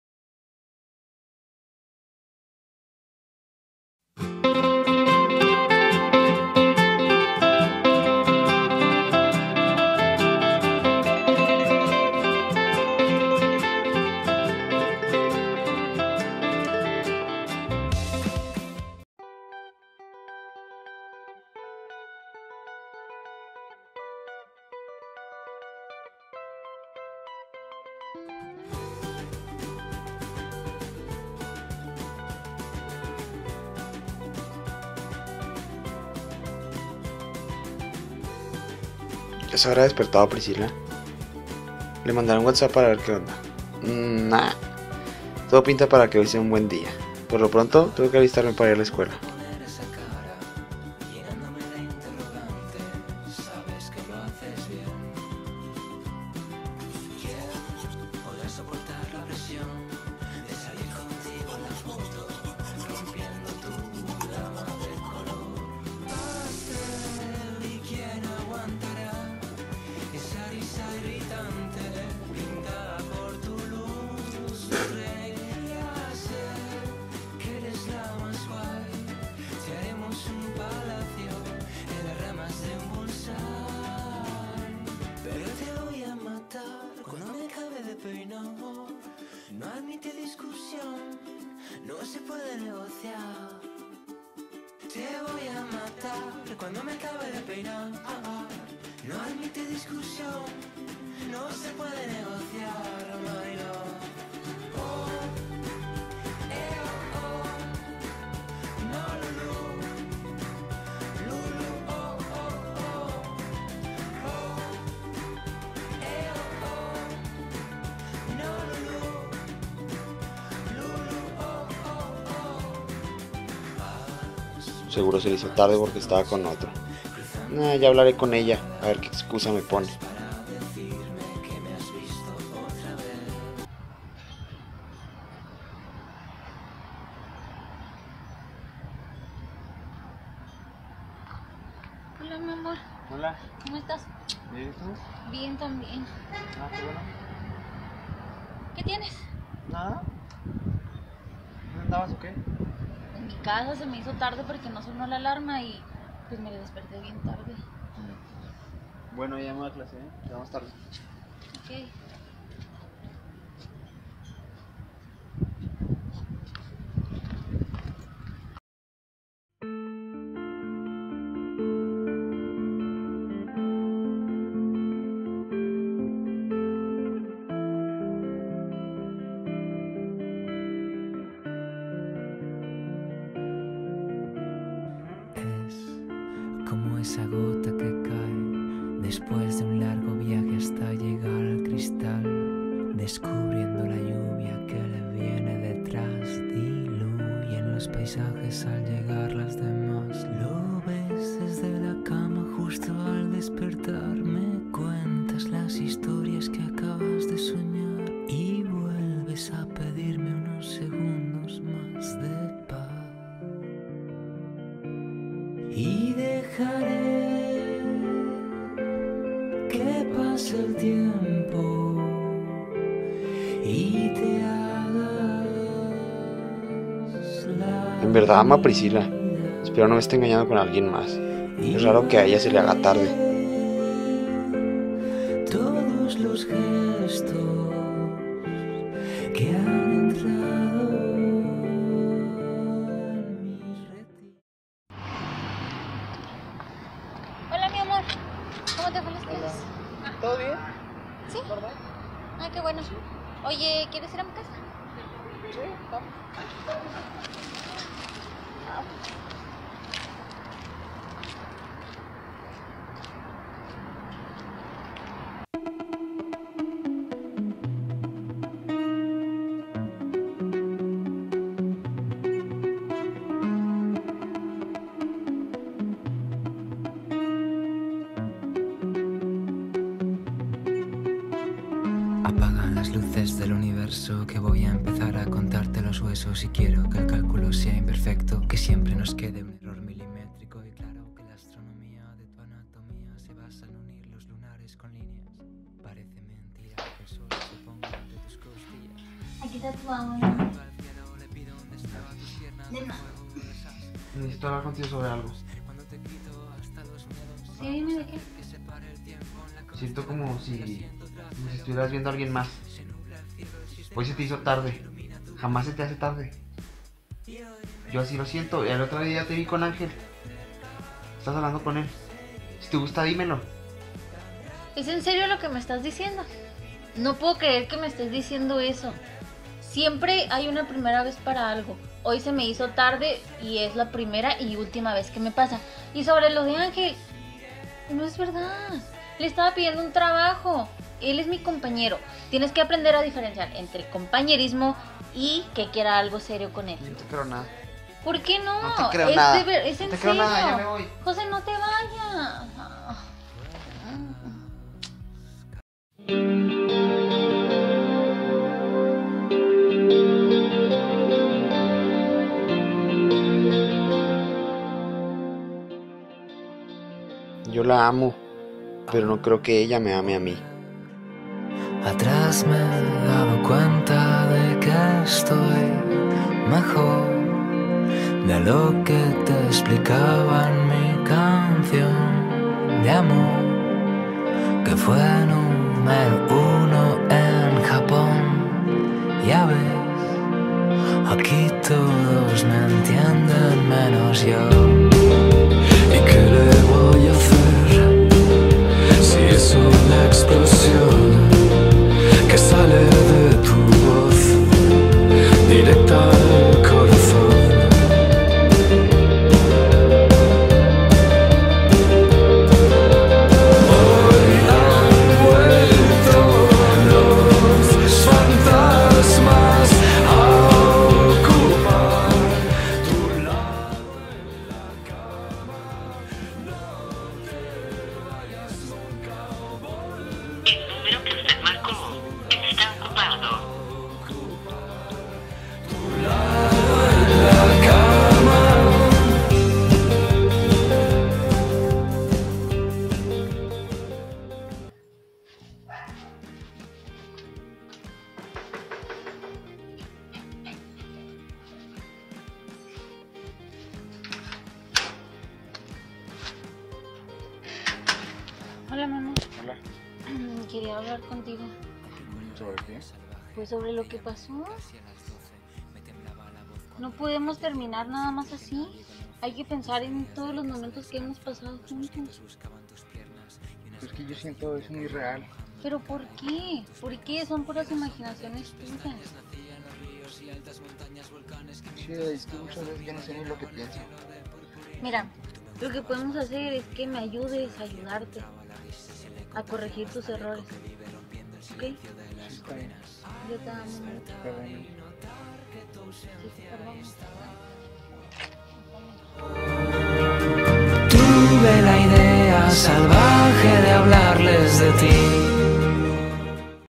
Perfect Imperfections | Cortometraje
Cortometraje realizado para la clase de Producción de medios audiovisuales.

Facultad de Ciencias Políticas y Sociales de la Universidad Autónoma de Chihuahua.

Este video no tiene fines de lucro.

Derechos de las canciones reservados por Carlos Sadness.